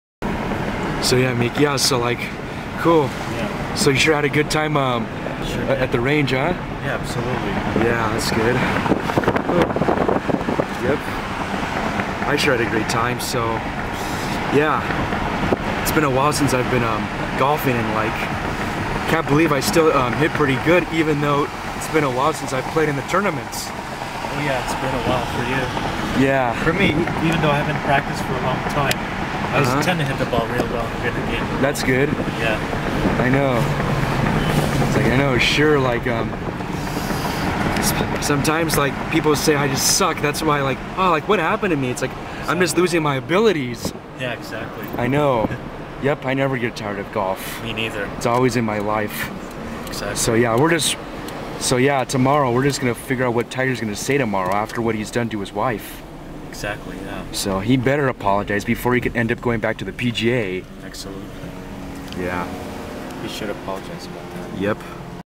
Riding with Mike
Riding in Mike's car and heading towards West Covina.   I decided to film this while Mike was driving back home.